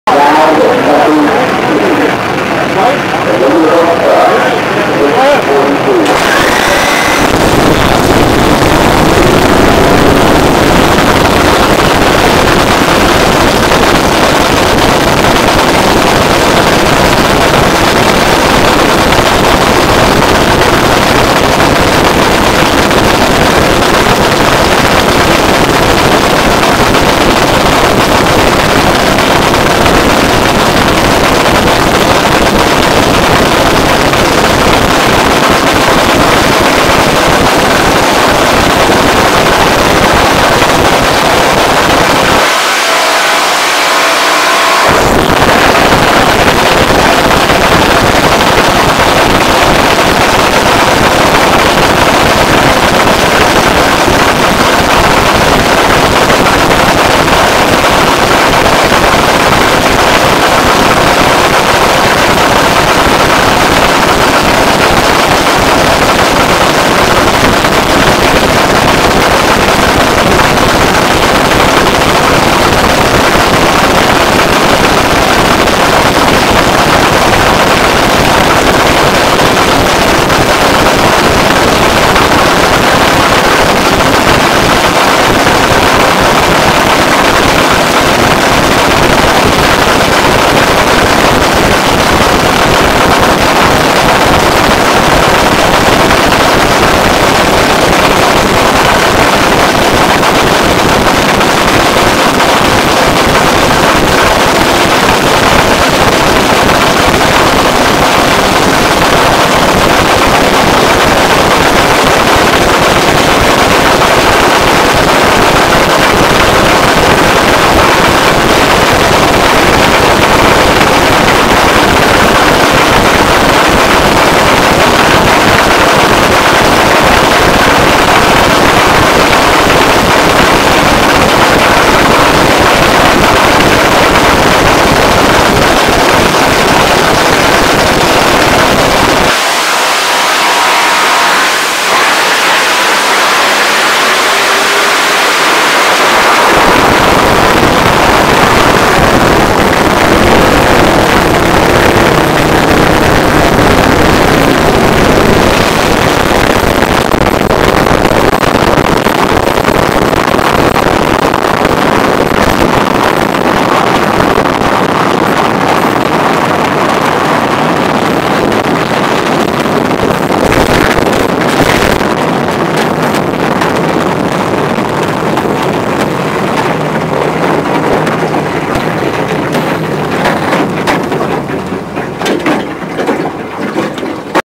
HSMS Bowling Green NHRR #1 Qualifier 5.89 @ 254 mph
Brad Thompson and the High Speed Motorsports team at the 2010 National Hot Rod Reunion June 18-20, 2010.  This pass was their #1 qualifying pass, the car ran 5.89 at 254 mph.  This years win adds to the combined 7 wins of this race for the team.  The HSMS car dominated all weekend, Low ET, Top Speed, #1 Qualifyer and Top Fuel Champion!